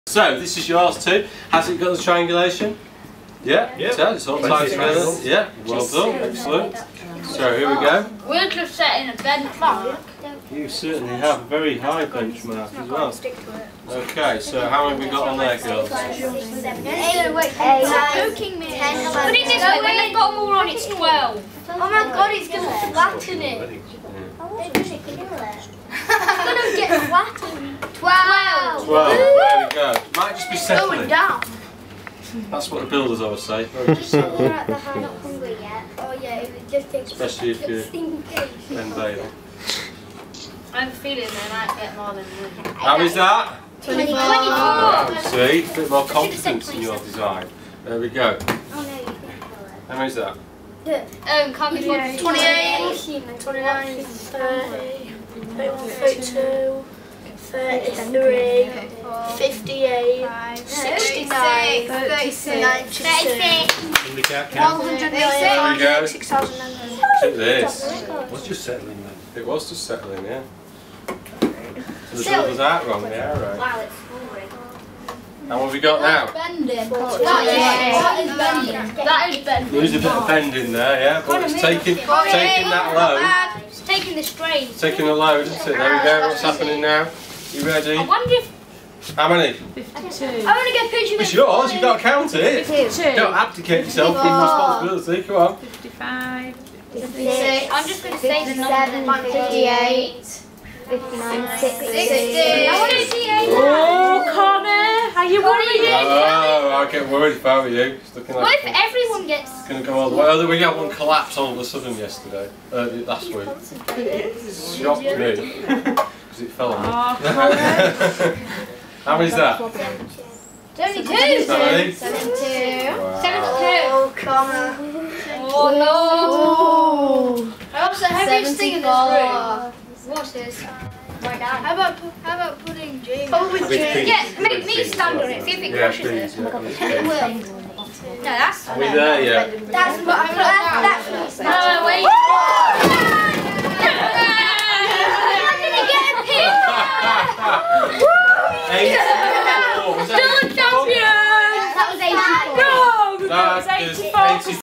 Tested to destruction!
Brigg Primary School pupils visited us today as part of the Corus Goal Challenge, focusing on the construction of a stable structure made from paper and glue. This video shows just how incredibly strong well-made structures can be, but demonstrates that every structure has a limit. This one was truely TESTED TO DESTRUCTION!